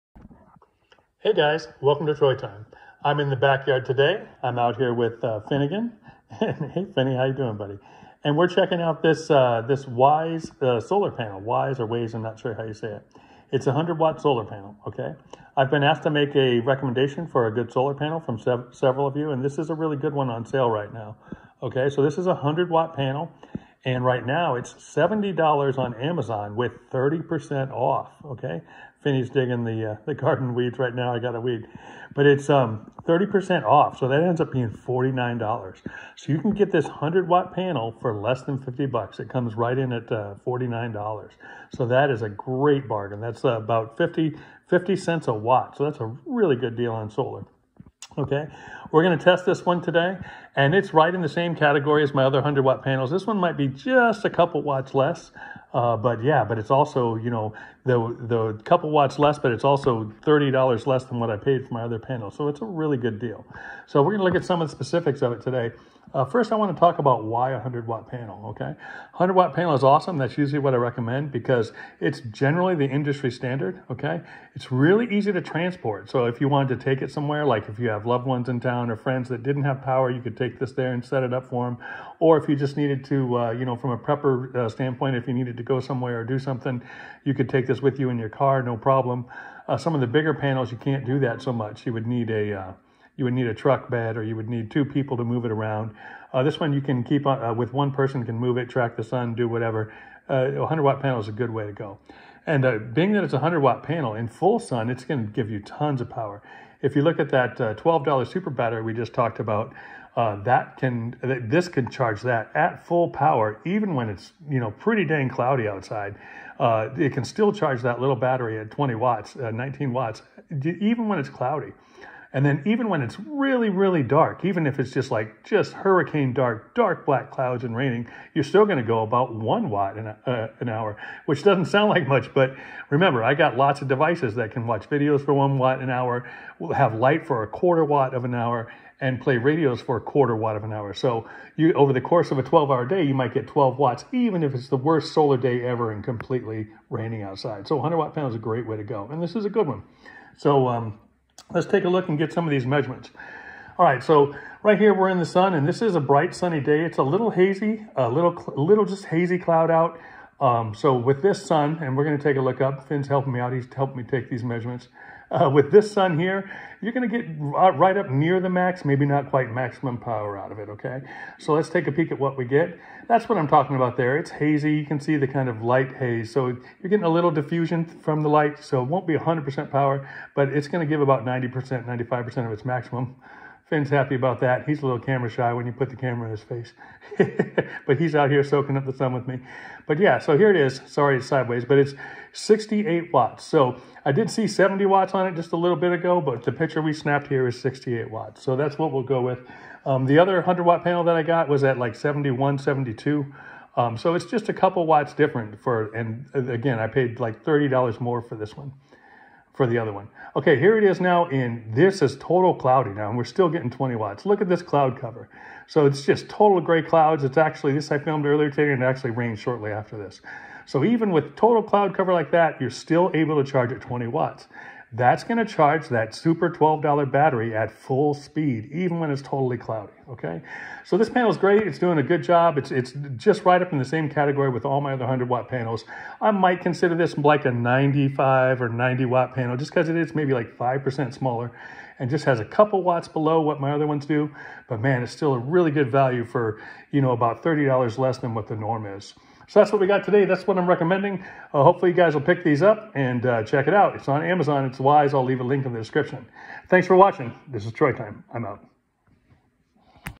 100W Solar panel for $49!!! I test and review it on a Sunny and Cloudy day!
Hey Guys! Here I test the Wieze 100W solar panel you can get on Amazon for $49!!! I show it both on a sunny day and on a cloudy afternoon. Be careful on Amazon because many panels claim to be 100W panels but really, they are MUCH less powerful. This one performed quite well. I recommend picking one up. It's a great deal at that price. Check out the video. Thanks :)